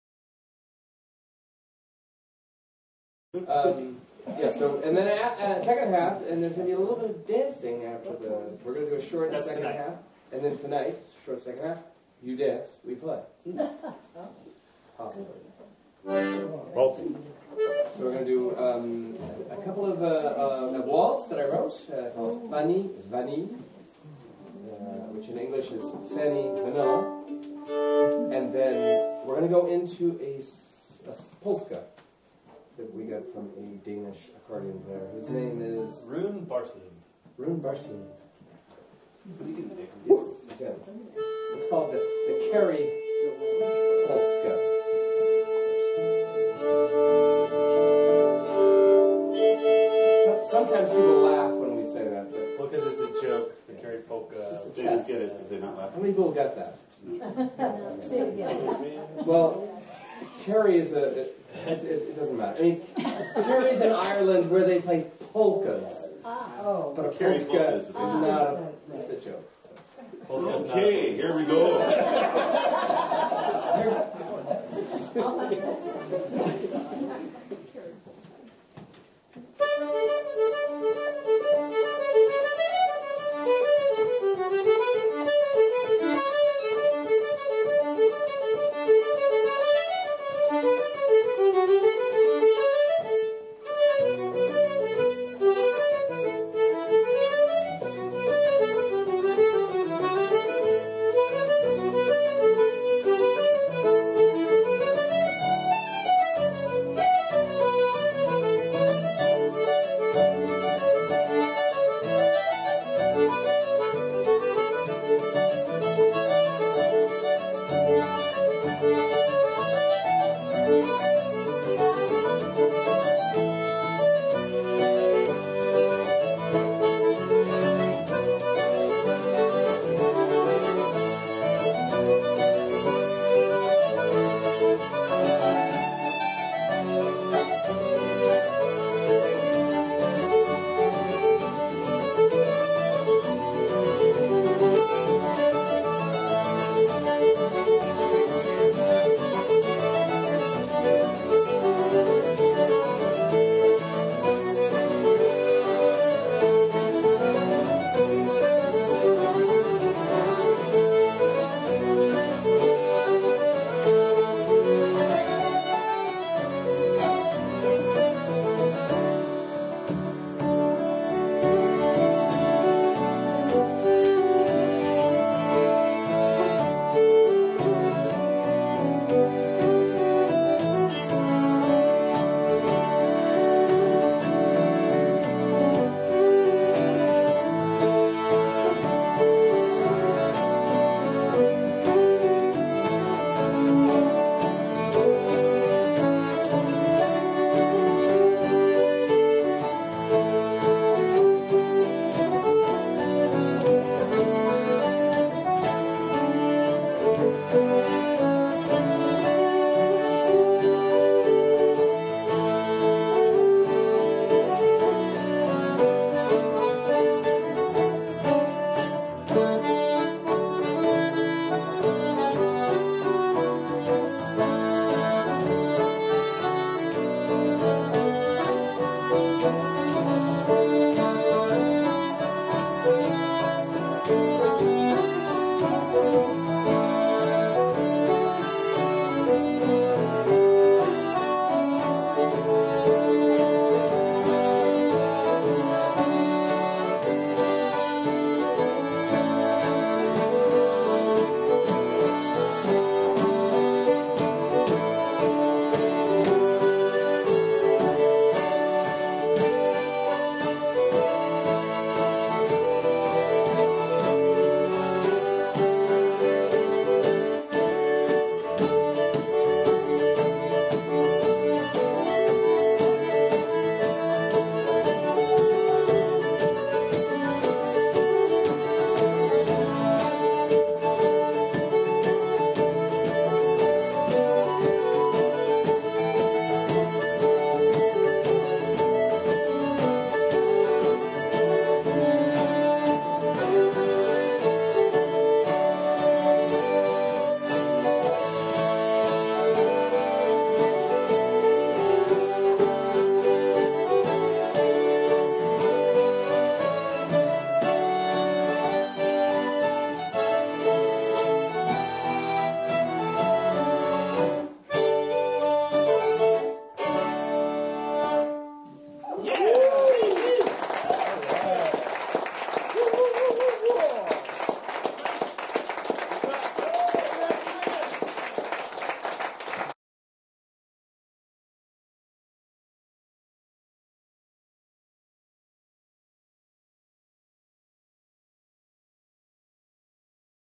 Kalos in a Sacramento House Concert November 2019
This video is about a great house concert by Kalos (Ryan McKassen, Eric McDonald and Jeremiah McLane) at Jimmy's in Sacramento in November 2019.  Wow are they great!  For more information on how you can help support these fantastic musicians during this time of social distancing when they can no longer play live for dances, go

Also note that Jeremiah McLane house (and instruments, including the accordian he is playing in this video) burned down in a fire last month.